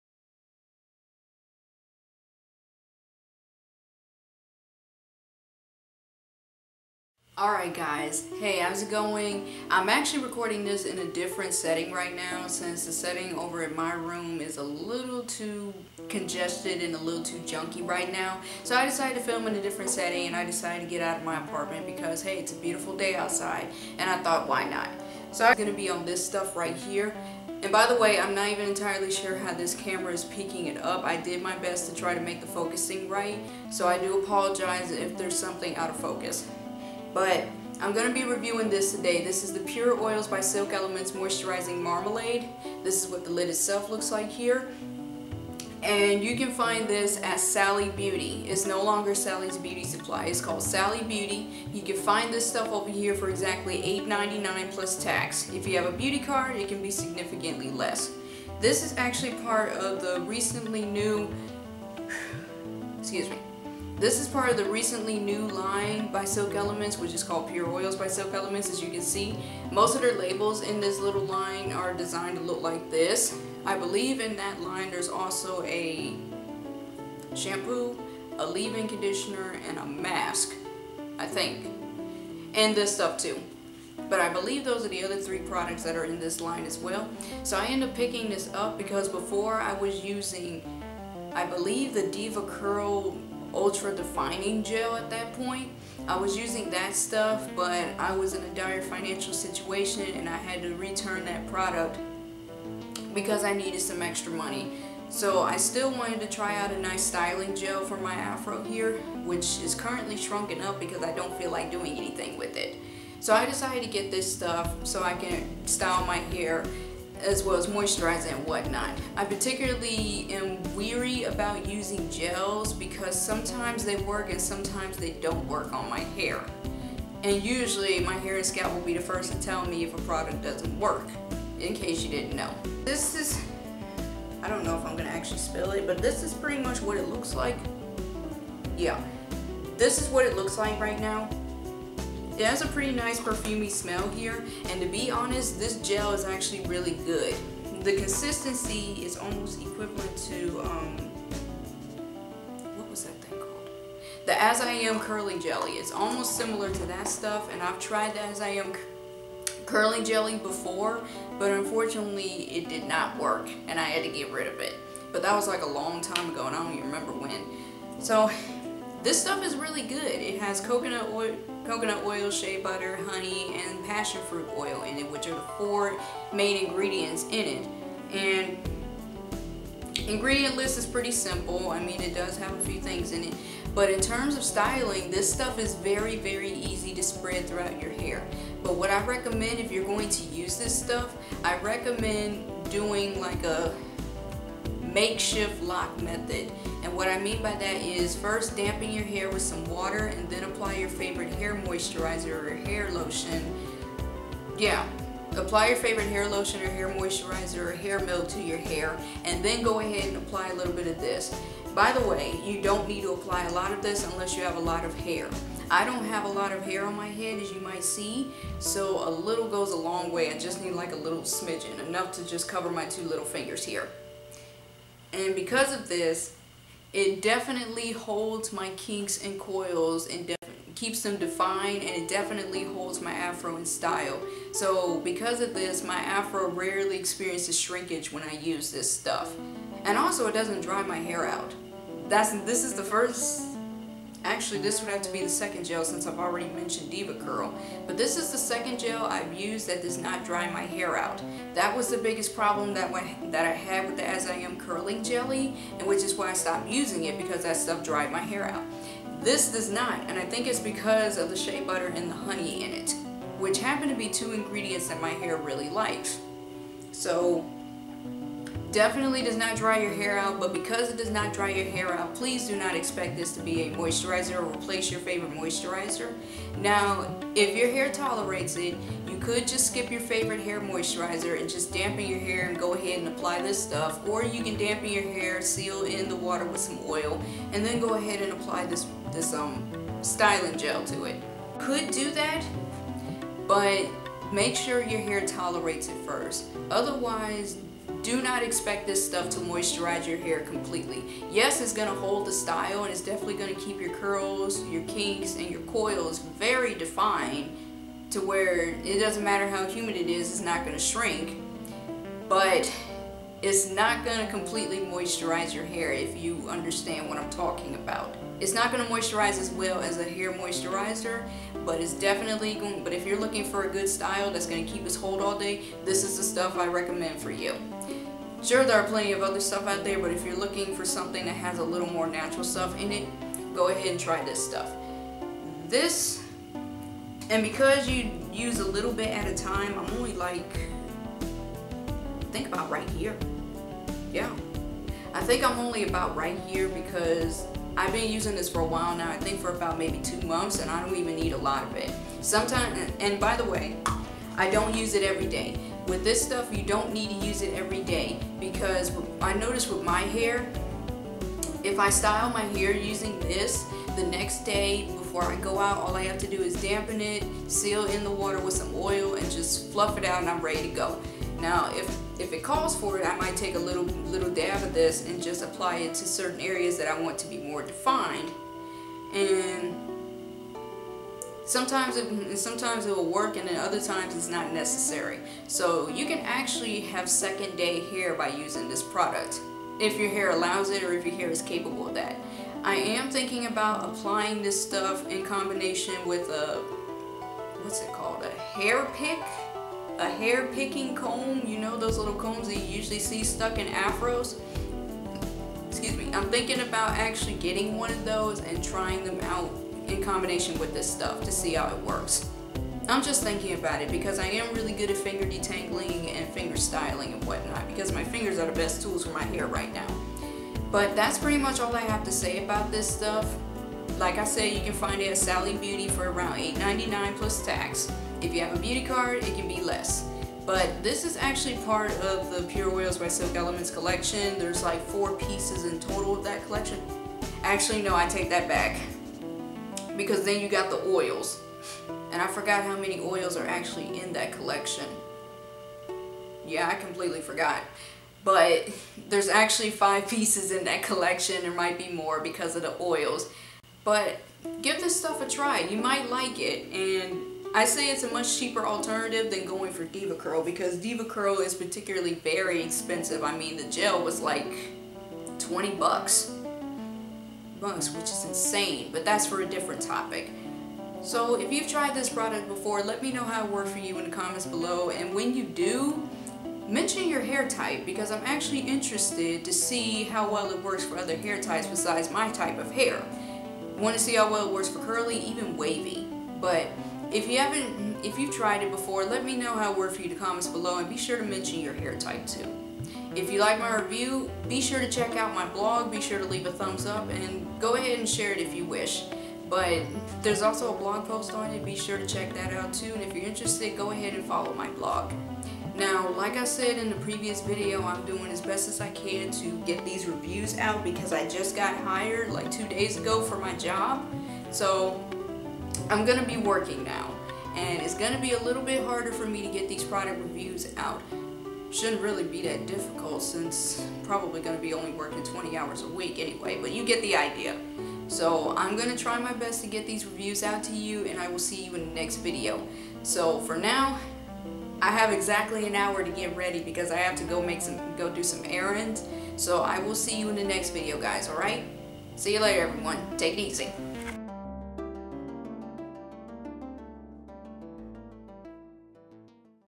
Review | Pure Oils by Silk Elements Moisturizing Marmalade
Here's my review of the Moisturizing Marmalade from Pure Oils by Silk Elements! It can be found at Sally Beauty for around $8.99 (less if you have a Beauty Card).

All questions and comments can be left in the Comments below! Also, mention your hair type if you've used it before!